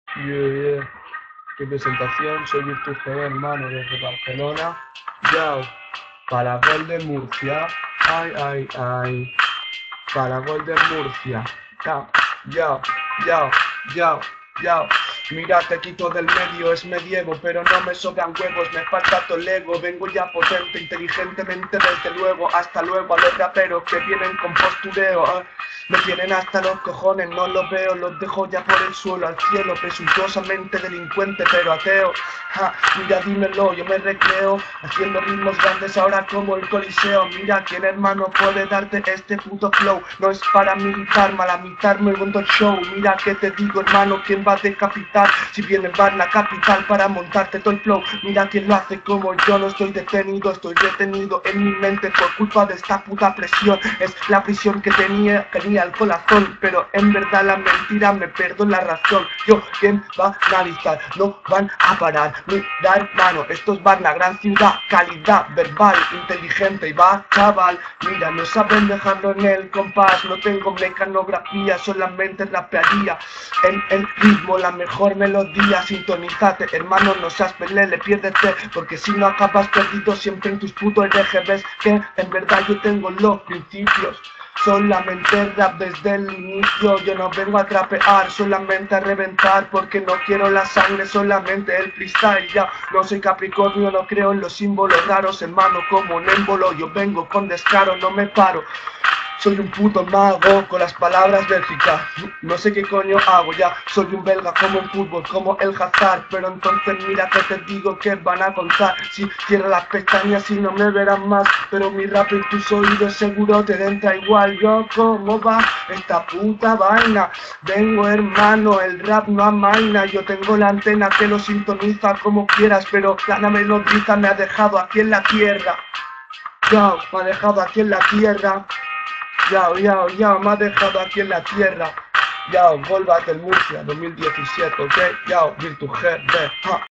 Gold Battle Murcia - Enric González
Esta es mi candidatura para la Gold Battle de Murcia. Participa a través de la plataforma